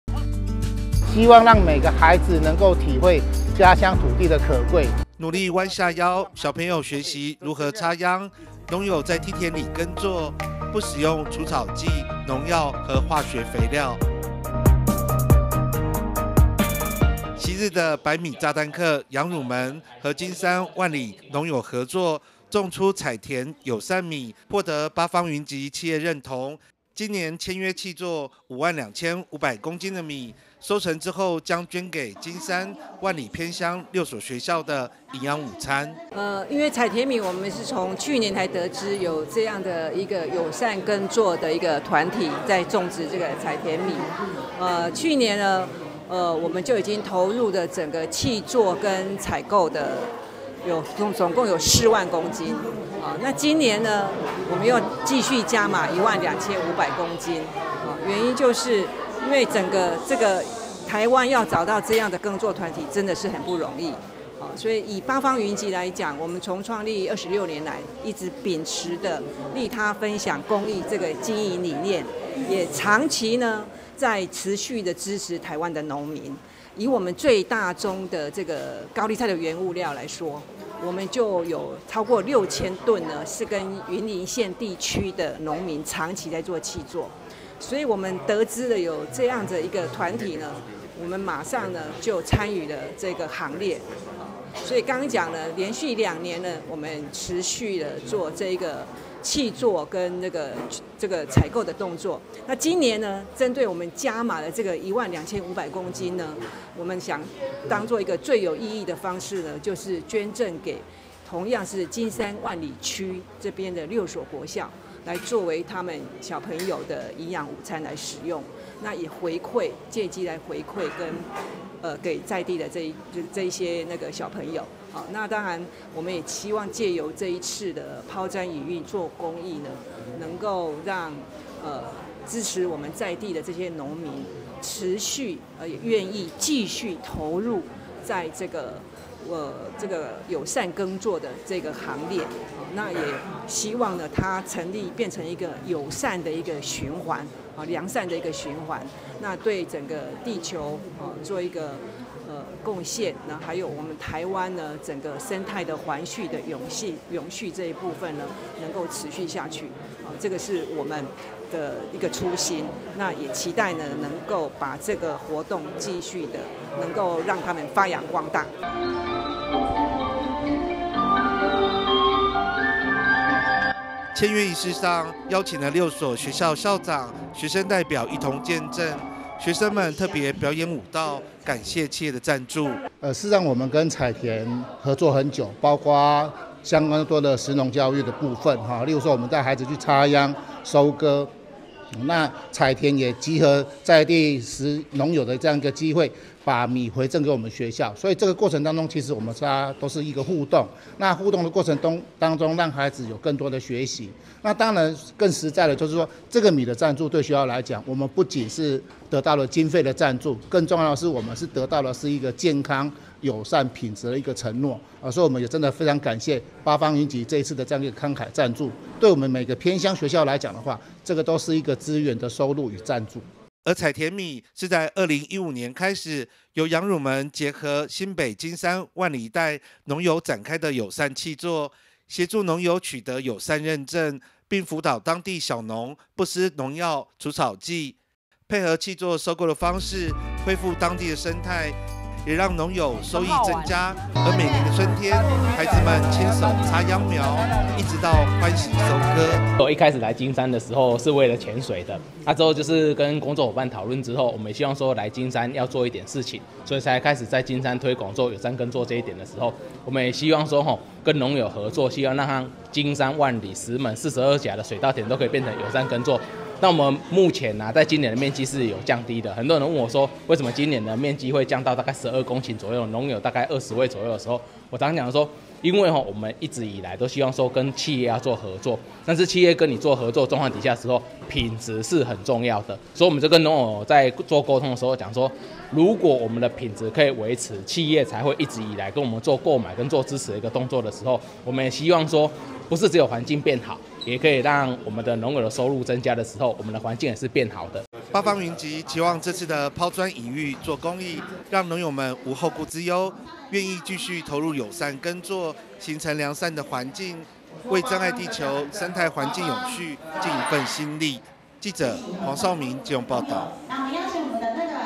農作｜八方雲集｜力挺友善農作！八方雲集捐贈偏鄉國小營養米
(中嘉新聞記者黃紹銘／基隆採訪報導)八方雲集企業與金山「彩田米」農友合作契作，今年簽約契作5萬2千5百公斤的米，將八月秋收後捐贈給新北市金山萬里偏鄉共六所國民小學營養午餐，幫助台灣農業朝向友善耕作發展，讓台灣農業友善永續，環境變好，孩童知道所吃的米飯是哪來的。

努力彎下腰，小朋友學習如何插秧，農友在梯田裡耕作，農友不使用除草劑、農藥及化學肥料。
昔日的白米炸彈客楊儒門，與金山、萬里農友合作，種出"彩田友善米"，獲得八方雲集企業認同，今年簽約契作五萬二千五百公斤米，收成後將捐給金山、萬里偏鄉六所學校營養午餐。
簽約儀式上，邀請六所學校校長、學生代表一同見證，學生特別表演舞蹈，感企業的贊助。
而「彩田米」是2015年開始，由楊儒門結合新北市金山、萬里一帶的農友開始友善契作，協助農友取得友善認證，並輔導當地小農不施農藥、除草劑、化學肥料等農作手法，配合契作收購的方式，恢復當地生態，也讓農民收益增加，每年春天，孩童親手插秧苗、一直到歡喜收割。
八方雲集期望這次拋磚引玉做公益，讓農友們無後顧之憂，願意繼續投入友善耕作，形成良善的循環，為珍愛地球，生態環境永續盡一份心力。